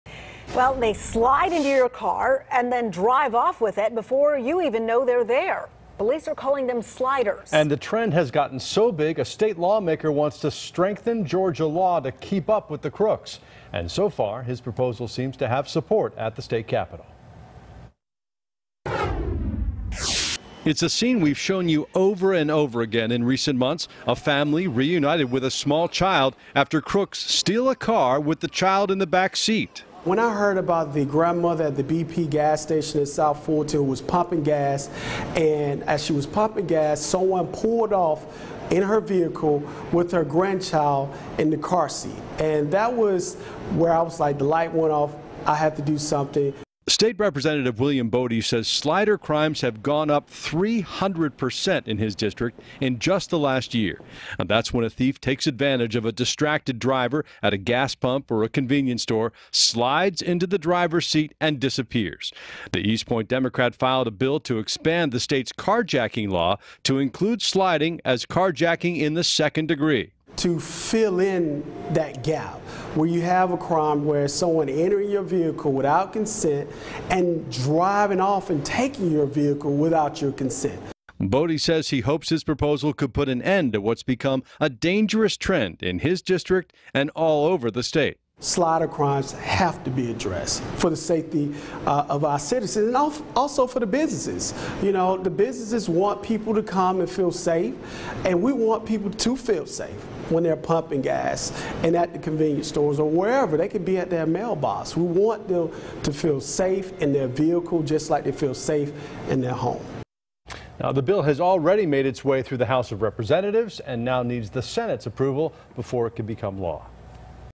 Georgia lawmaker wants to classify 'sliding' as carjacking
Police call it "sliding," when a criminal slides into someone's car as they pump gas and drives away.  Now, a Georgia lawmaker wants to update the state's carjacking statute to include "sliding."

Representative William Boddie, D- East Point, filed House Bill 67, which would create two distinct categories of hijacking a motor vehicle. Criminals who use a weapon or force or intimidation to steal a car would be charged with first degree carjacking. The second degree would be reserved for those who take a vehicle without necessarily interacting with their victim.

WATCH: Hear what lawmakers have to say about the bill in their own words
IFrame

"[The bill is designed] to fill in that gap where you have a crime where someone enter your vehicle without your consent and driving off and taking your vehicle without your consent," Rep. Boddie explained.

Boddie said he was inspired to file legislation to address the issue after hearing about a grandmother whose car was stolen in his district with her grandchild in the back seat.

"Slider crimes have to be addressed for the safety of our citizens and also for the businesses," said Rep. Boddie. "You know, the businesses want people to come and feel safe and we want people to feel safe when they're pumping gas and at the convenience store or wherever. They could be at their mailbox. We want them to feel safe in their vehicle just like they feel safe in their home."

Residents got the chance to hear about the proposal at a Wednesday night town hall in Rep. Boddie's district at Westlake High School. They tell FOX 5, although many feel relatively safe at a gas station, they still take an abundance of caution.

WATCH: Residents voice their concerns over 'sliders'
IFrame

"I am always aware-- looking around to make sure," said one resident.

Fulton County Police said slider crimes have dropped significantly in 2017.

Members of the House of Representatives approved House Bill 67 last week and it is now up to the Senate to review the measure.

NEXT ARTICLE: Two school bus crashes in Forsyth County